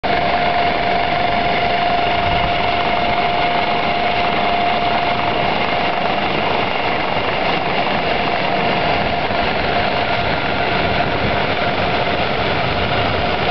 Coast Guard protecting the QM2
Making sure no one jumped onto the ship from the Golden Gate Bridge?